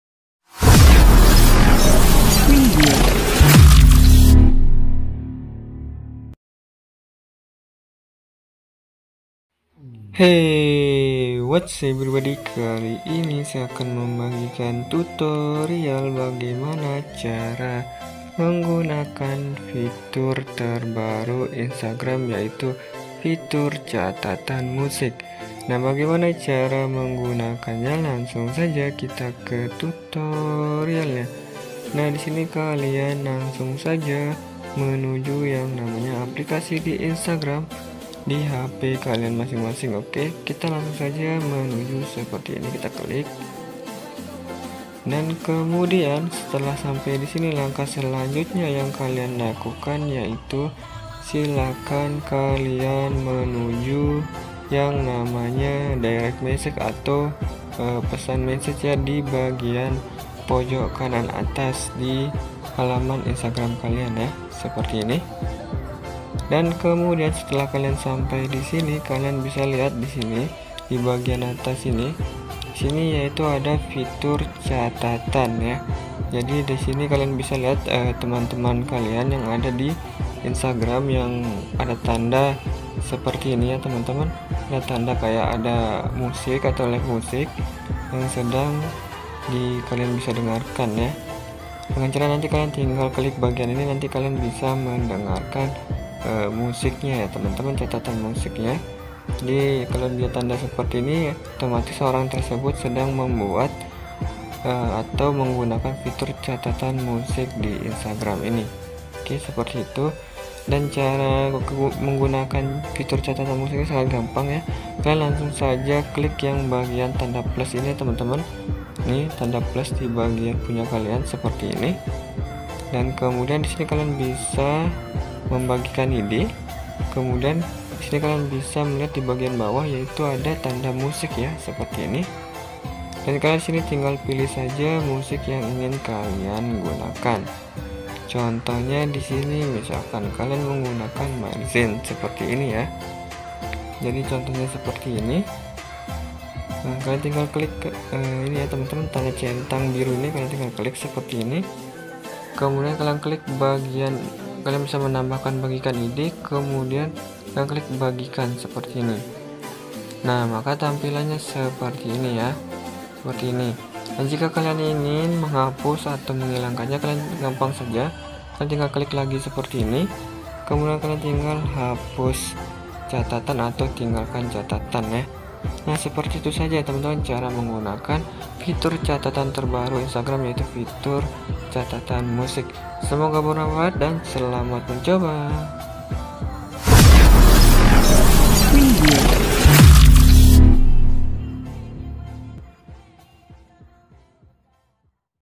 Cara Menggunakan Fitur Catatan Musik Di Instagram | fitur catatan lagu di instagram
Okee,,

Pada kesempatan kali ini saya akan membagikan video bagaimana cara menggunakan fitur catatan musik di instagram..

Selamat mencoba...